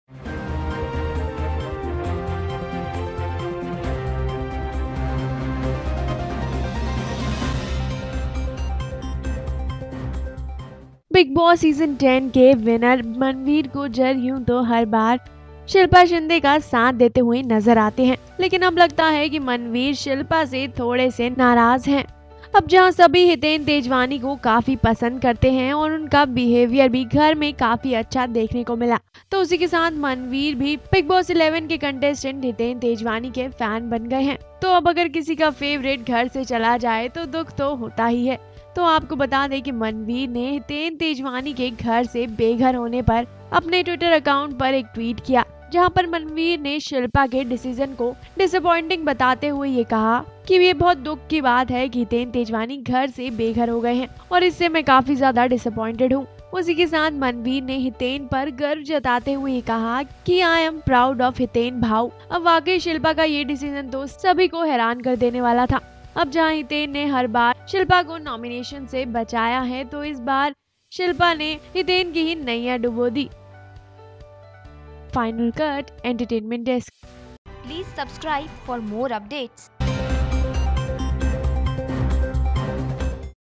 Maneer Gujjar Slams Shilpa For Hiten's Elimination|| Manveer On Shilpa || Bigboss 11
Manveer Gujjar On Shilpa Shinde.